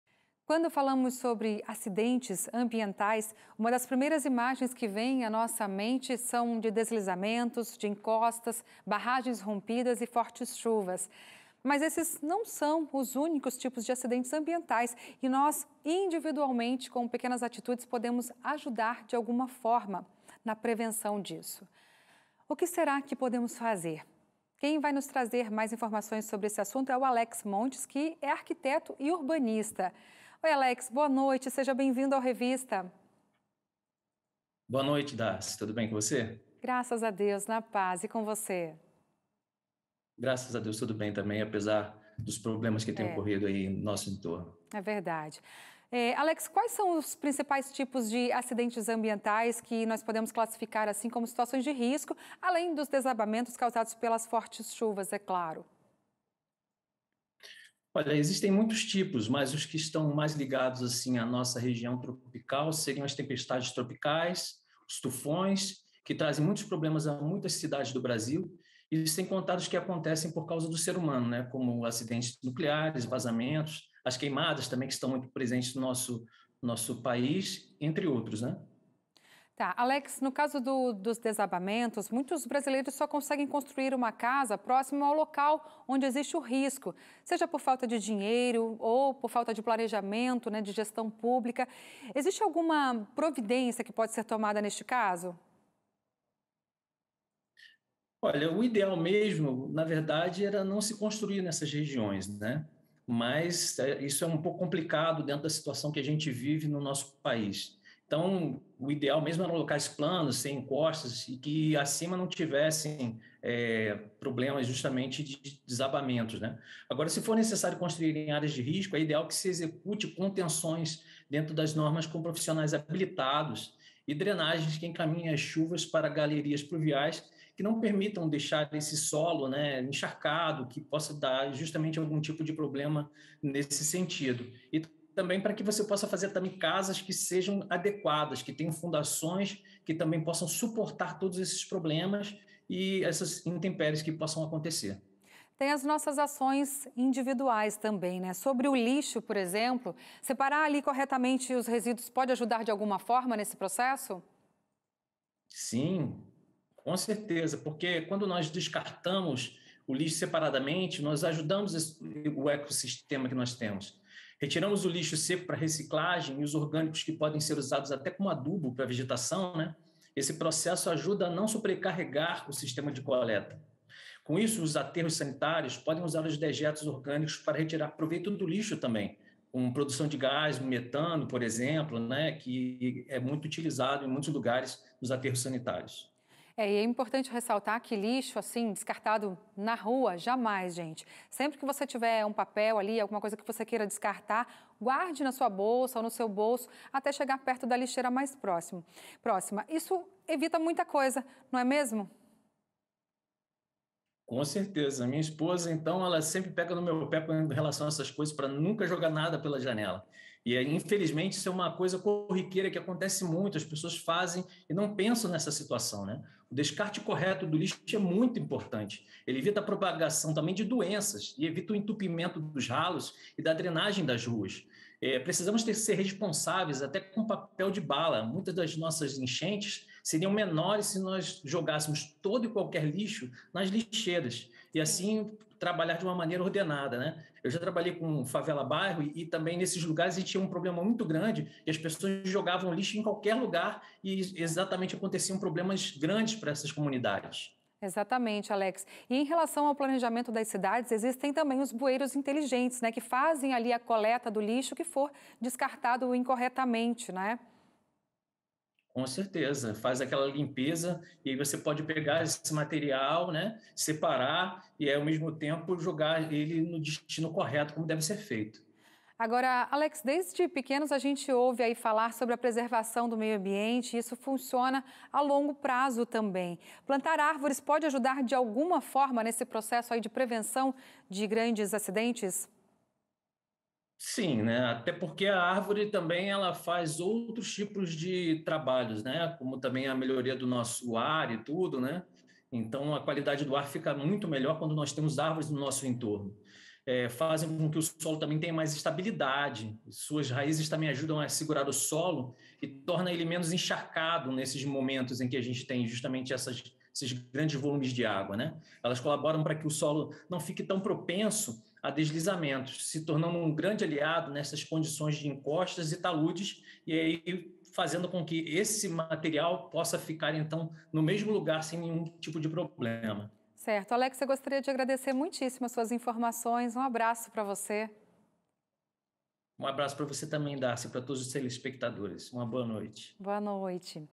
QUAL A SUA PARTICIPAÇÃO NOS DESASTRES AMBIENTAIS?
QUANDO FALAMOS SOBRE ACIDENTES AMBIENTAIS, UMA DAS PRIMEIRAS IMAGENS QUE VÊM À NOSSA MENTE SÃO DE DESLIZAMENTOS DE ENCOSTAS, BARRAGENS ROMPIDAS E FORTES CHUVAS. MAS ESSES NÃO SÃO OS ÚNICOS TIPOS DE ACIDENTES AMBIENTAIS E NÓS, INDIVIDUALMENTE, COM PEQUENAS ATITUDES, PODEMOS AJUDAR DE ALGUMA FORMA.